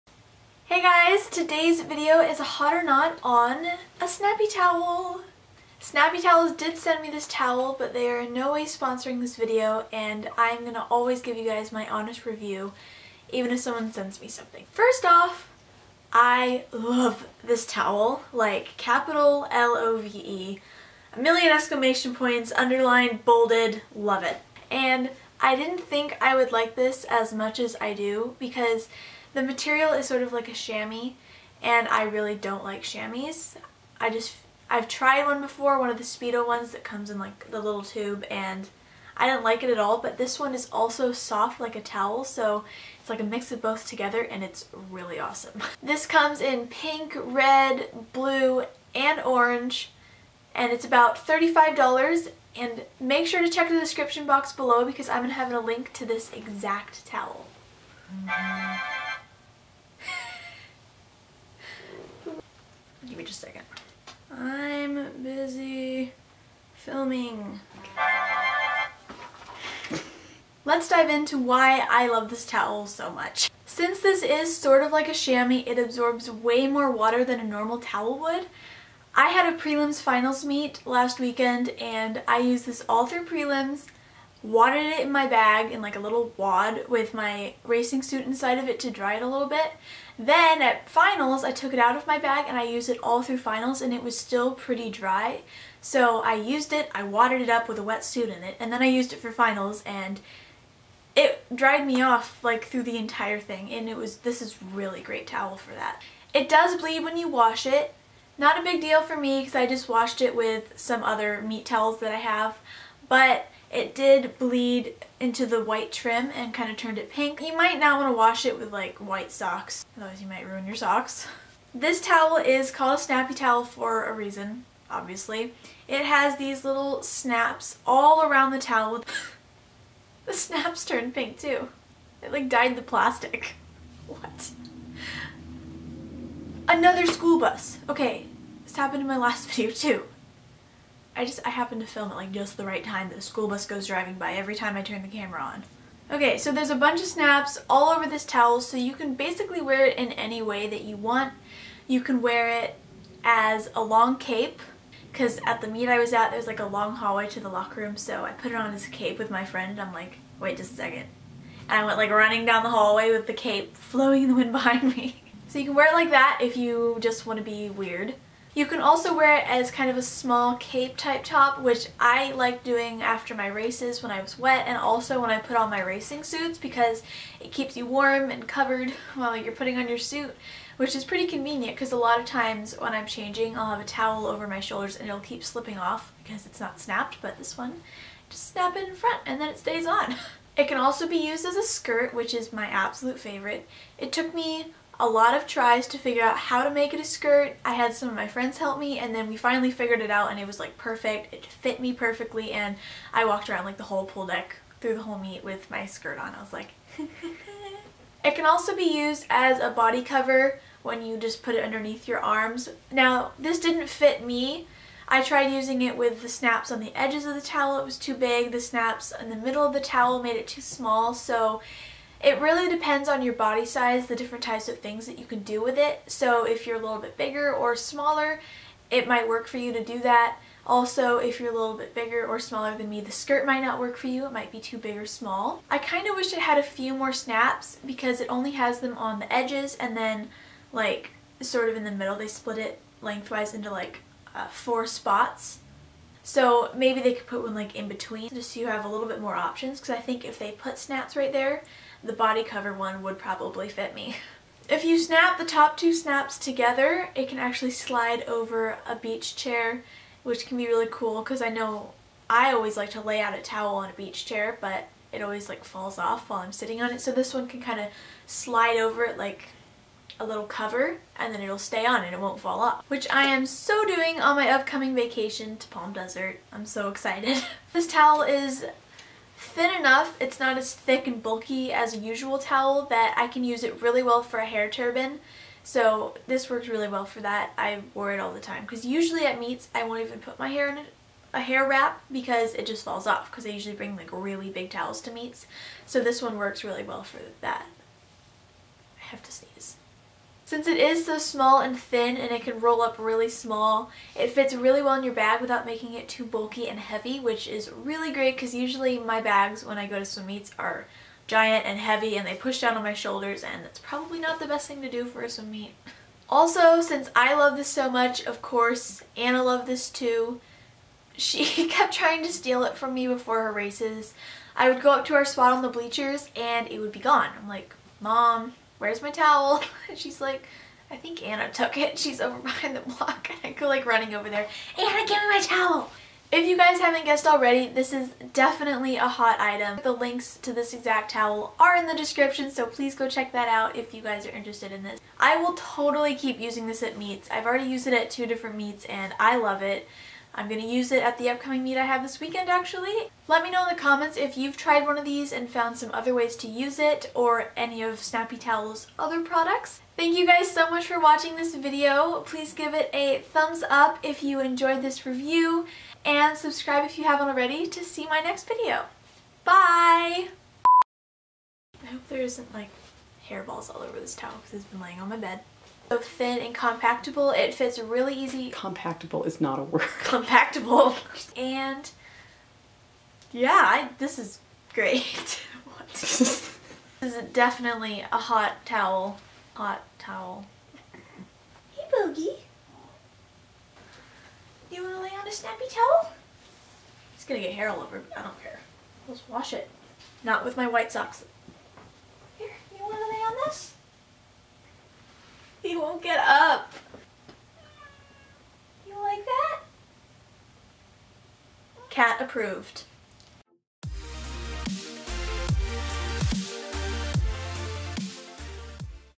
Snappy Towels Review | Hot or Not
Hey guys!  Today's video is a hot or not review of a Snappy Towel!  The company was nice enough to send me a towel for review.  This is my honest opinion and my opinion alone.  :)  I hope you enjoy!

This is the exact towel I talk about in my video.  It is greatly appreciated!!!